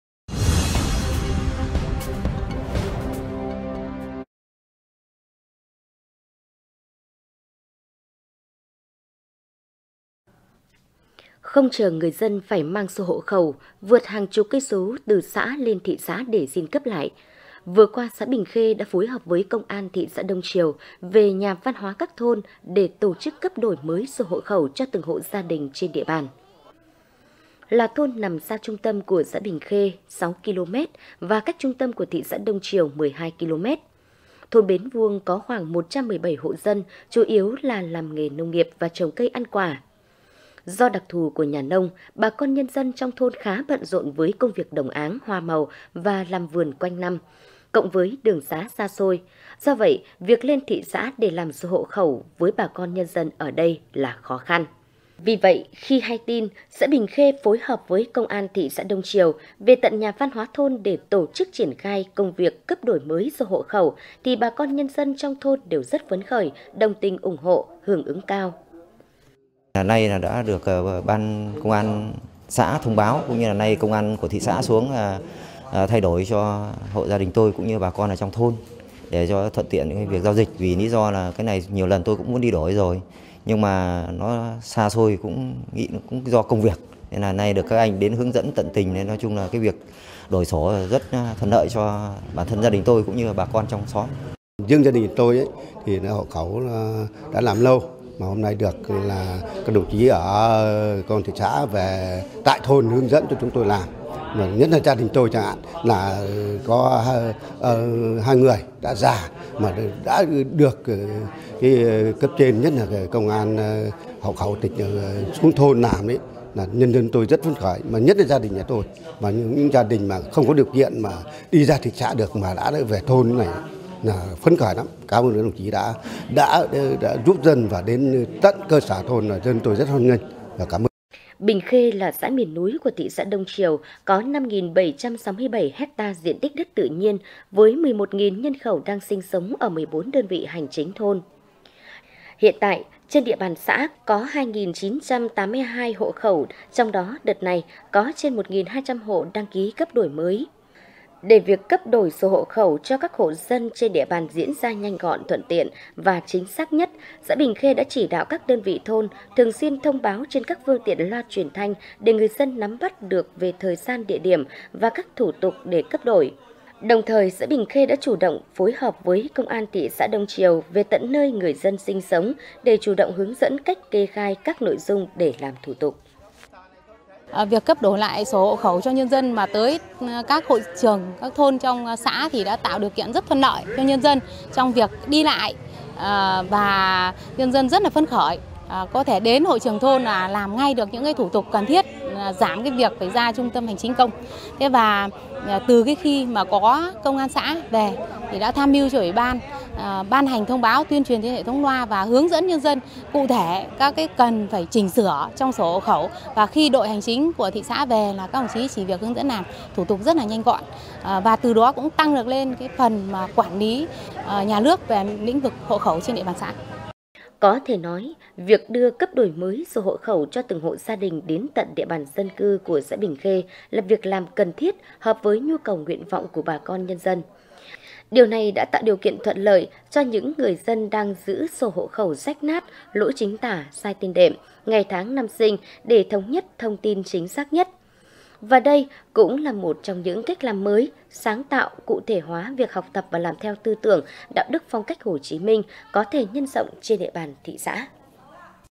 Đông Triều tv-Công an xã Bình Khê cấp đổi sổ hộ khẩu
Công an xã Bình Khê cấp đổi sổ hộ khẩu

(Đông Triều tv- kênh thời sự chính trị liên tục cập nhật)